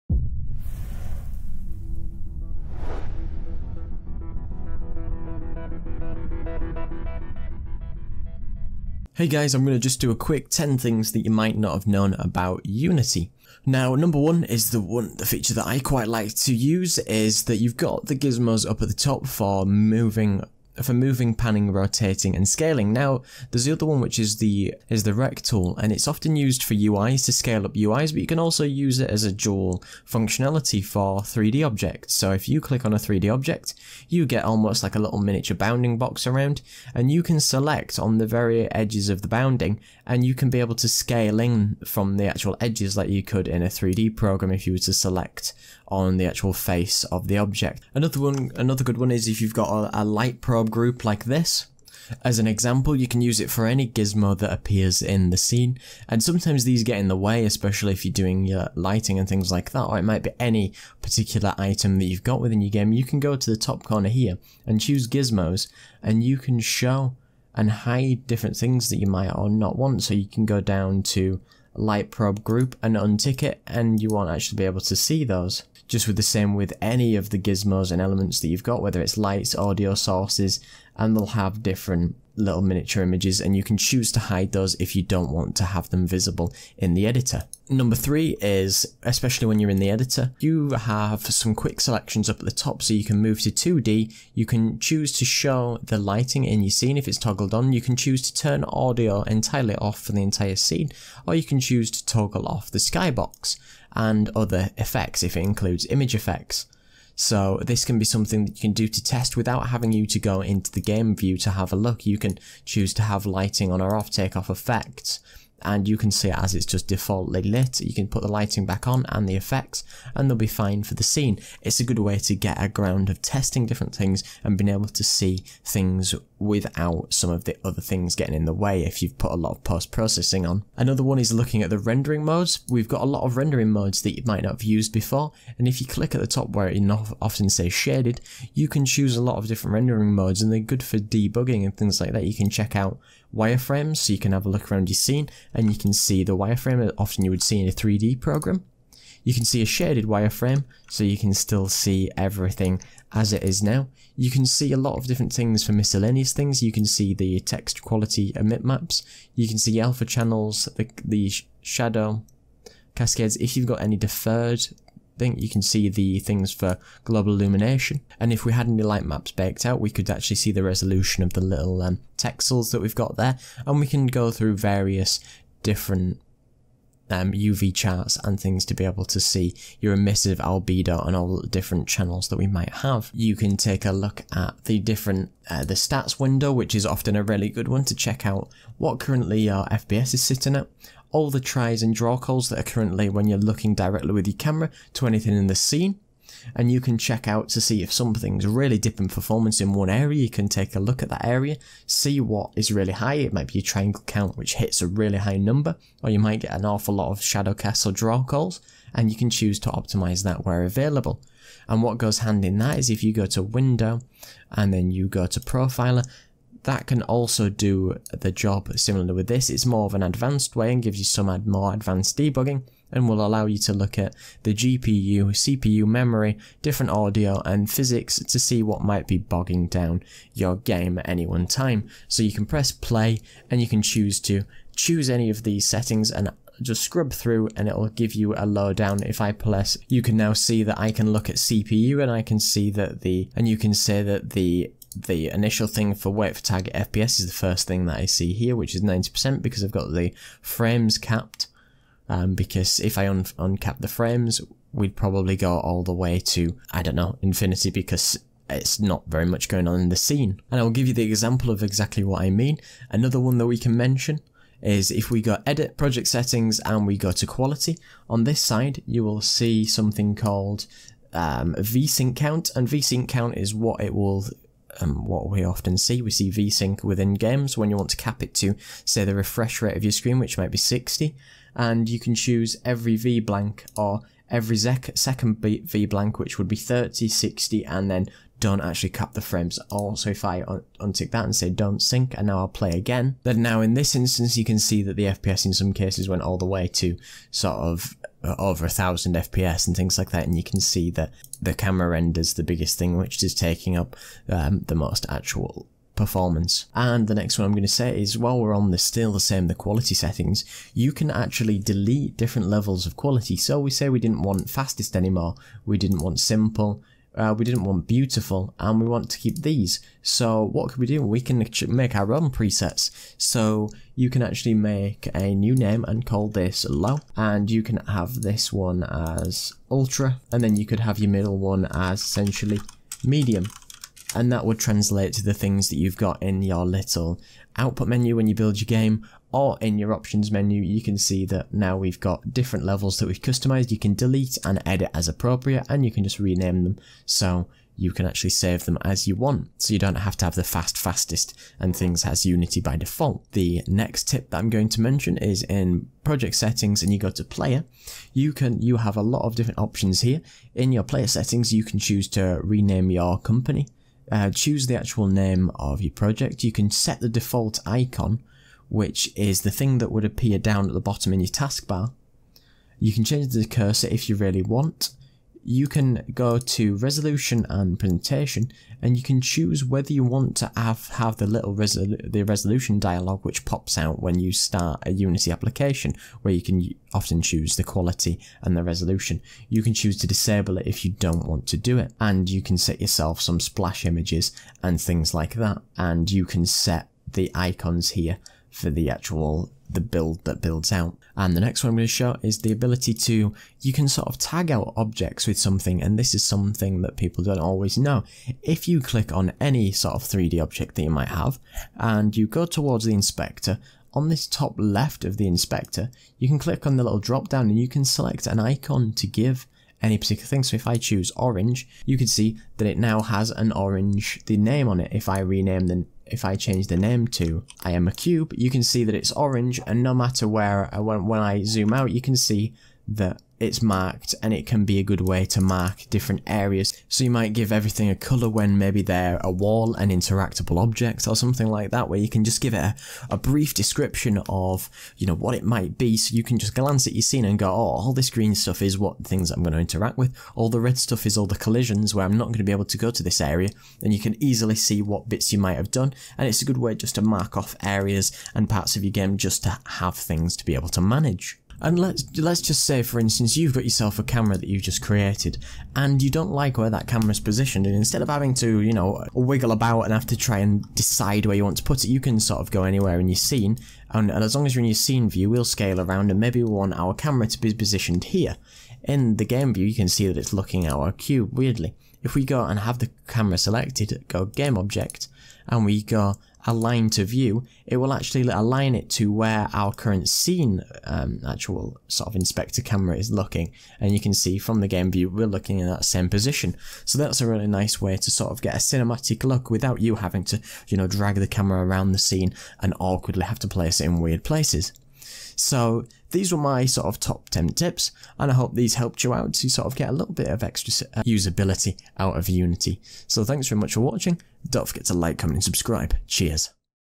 TOP 10 Unity TIPS - 2017
In this video I share my top 10 Unity tips for saving you time, effort and money in the long term! These little tips will save you a life time of headaches!